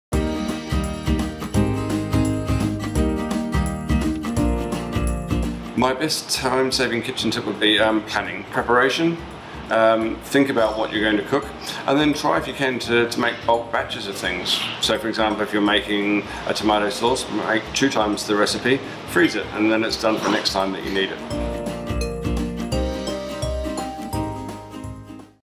Table Talk - L'atelier des Chefs Head Chef Q&A - Time Saving Kitchen Tip
Watch our sit down interview with the Head Chef from L'atelier des Chefs.

To celebrate our partnership with L’atelier des Chefs, Europe’s largest cookery school, Tefal recently invited a team of awesome foodie bloggers to cook lunch with us.